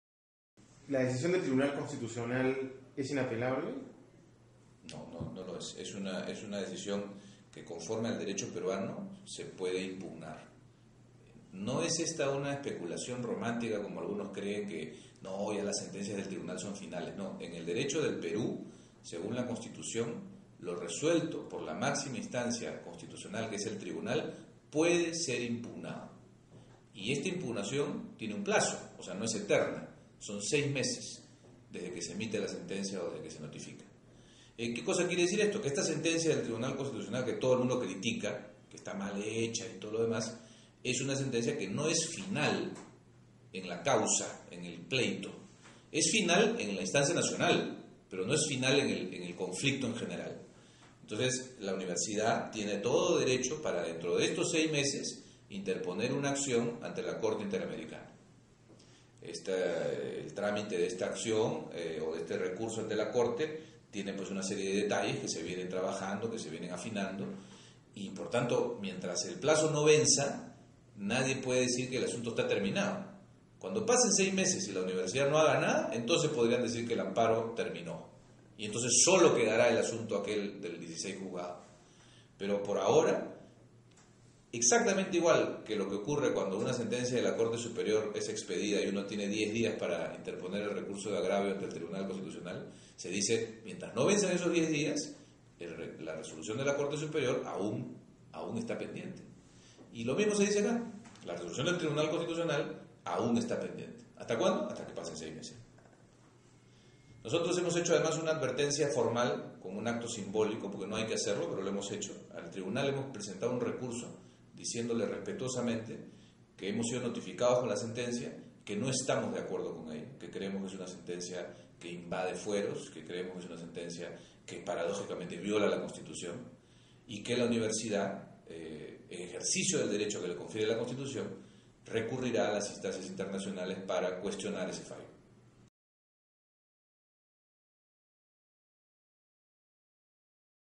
PUCP - Resolución del TC sobre el amparo se puede impugnar
La cuestionada sentencia del Tribunal Constitucional no será inscrita en registros públicos, ni tampoco será enviada por el TC al 16to Juzgado Civil, en el que se desarrolla el proceso para interpretar los testamentos de don José de la Riva-Agüero. Tribunal constitucional rechazó solicitudes del Arzobispado de Lima.